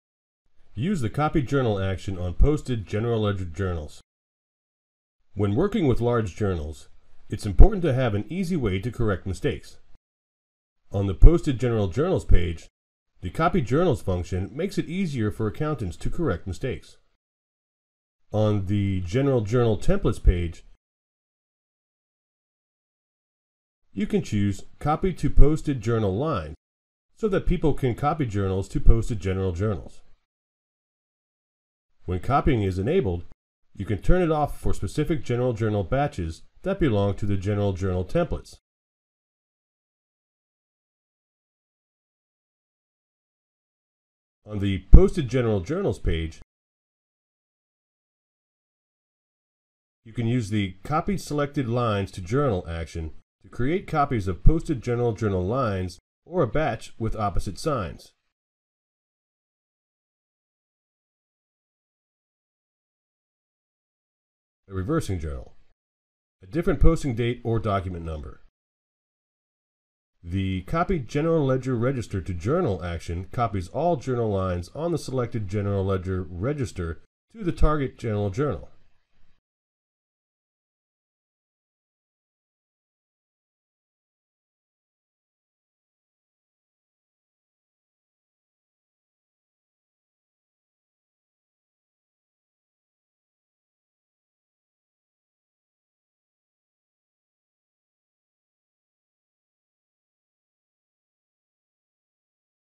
Copy journal function on Posted General Journals
In this feature overview video series, we highlight new capabilities included in the latest update to Dynamics 365 Business Central.

To speak with a Business Central expert, contact Admiral Consulting Group